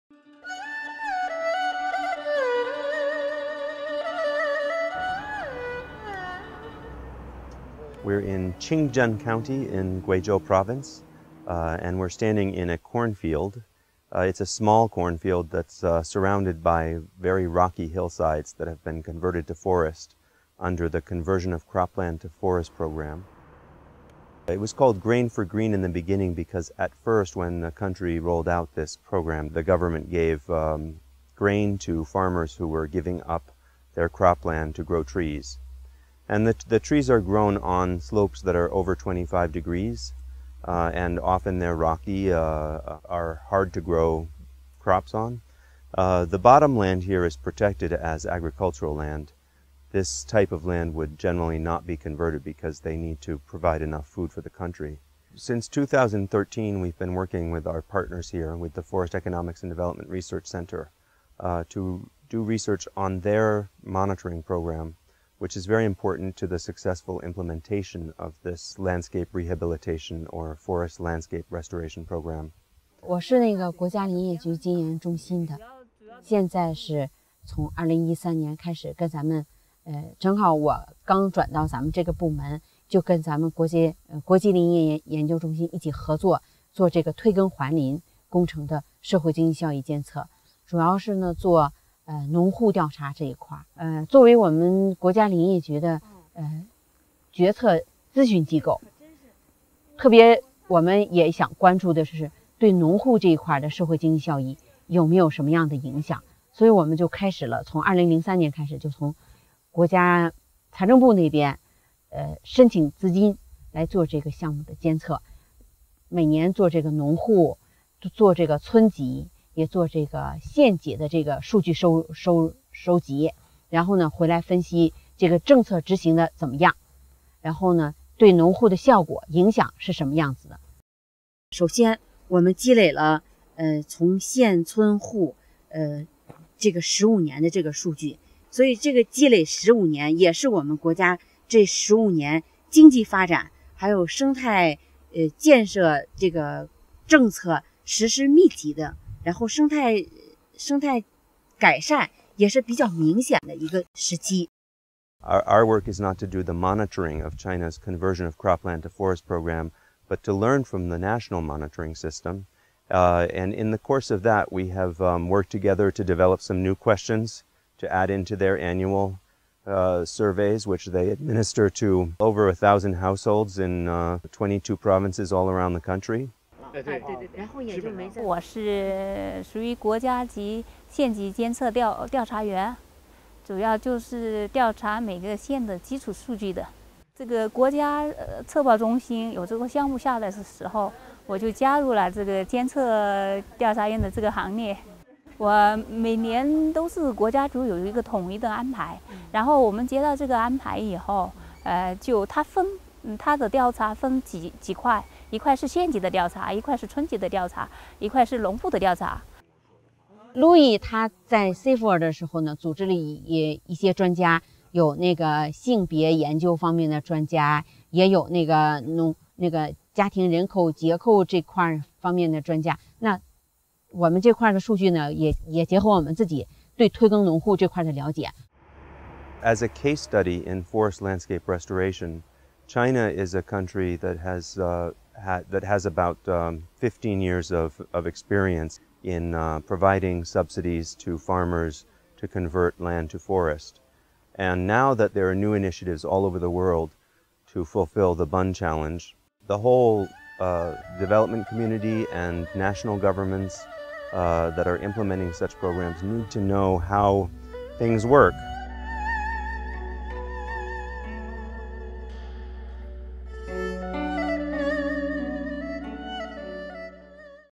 Restoring forest landscapes in China: Research and monitoring
The Chinese government has led a Conversion of Cropland to Forest Program since 1999. Research and monitoring of the program hopes to bring to light lessons for global Forest Landscape Restoration efforts, such as the Bonn Challenge.